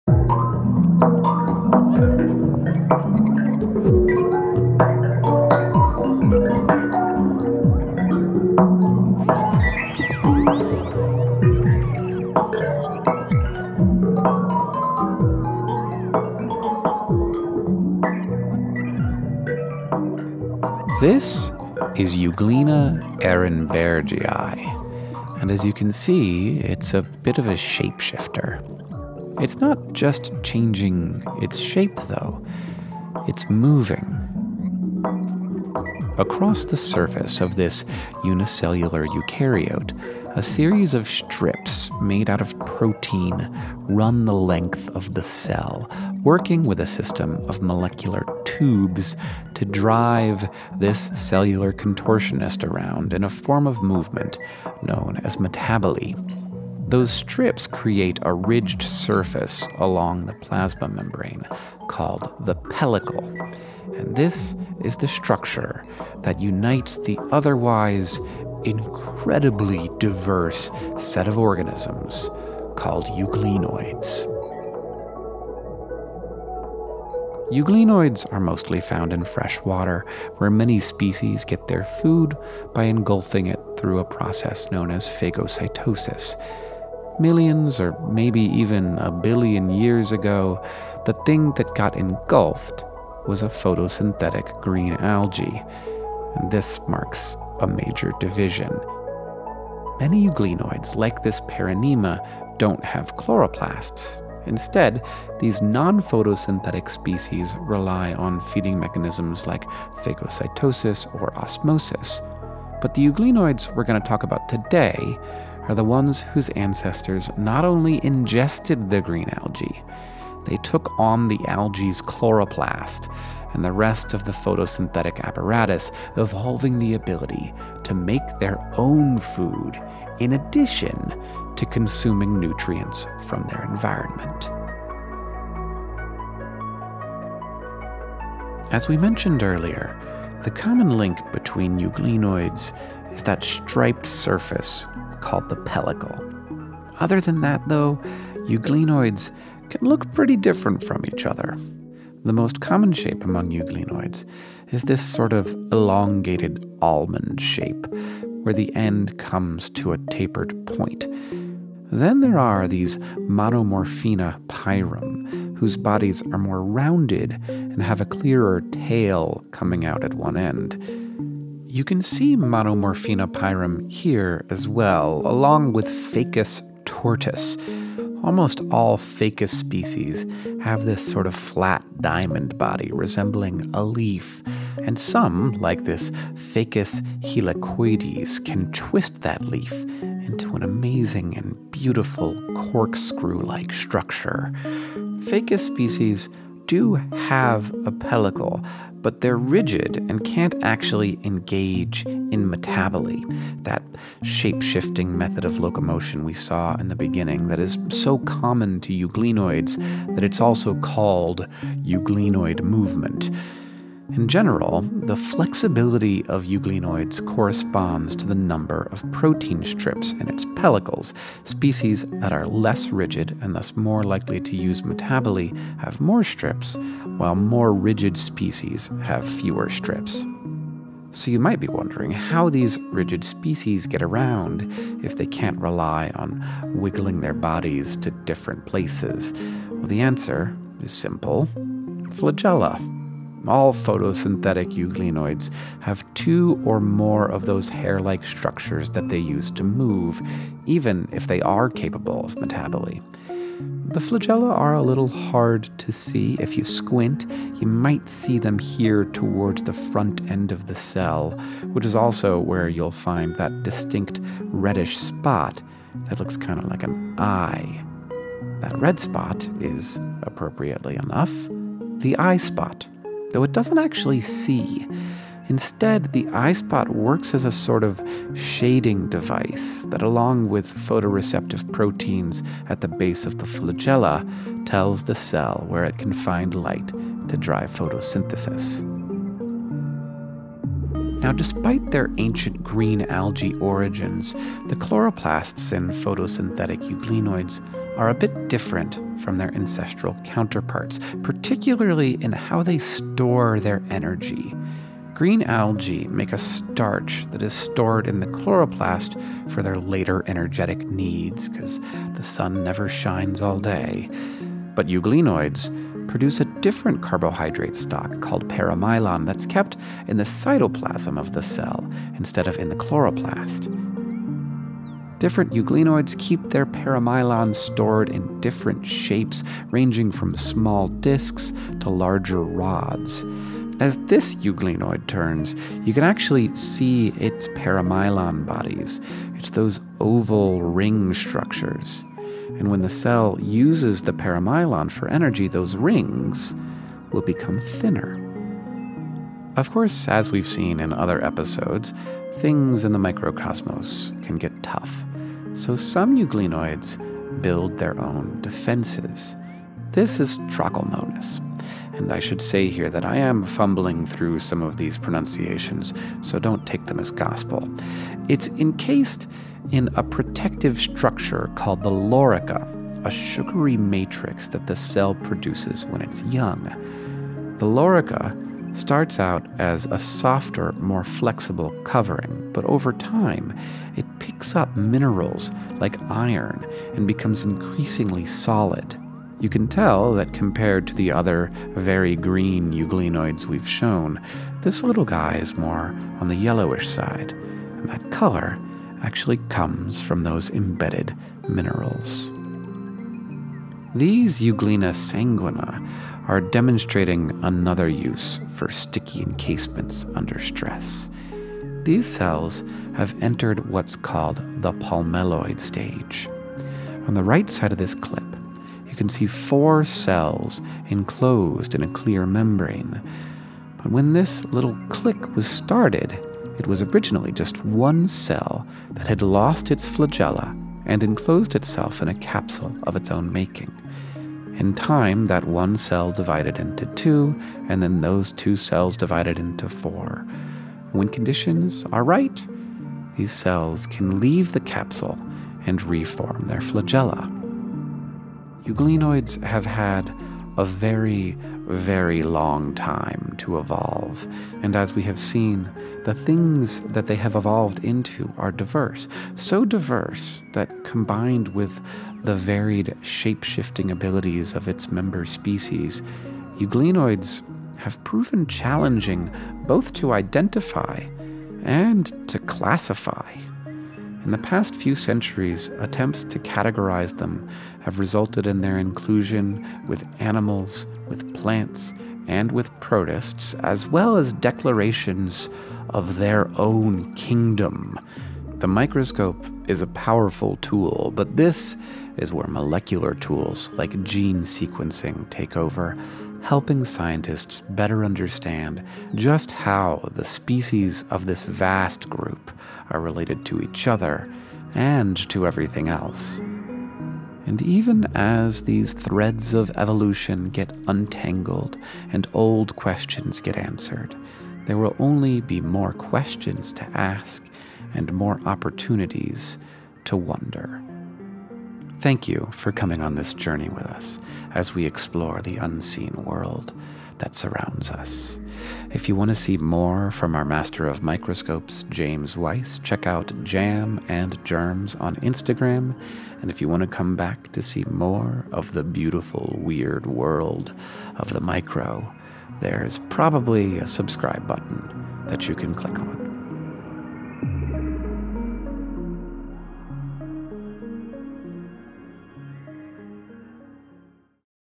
Euglenoids: Single-Celled Shapeshifters
Euglenoids have had a very, very long time to evolve, and that has led to the things they have evolved into being extremely diverse—so diverse that, combined with the varied shape-shifting abilities of its member species, euglenoids have proven challenging to both identify and classify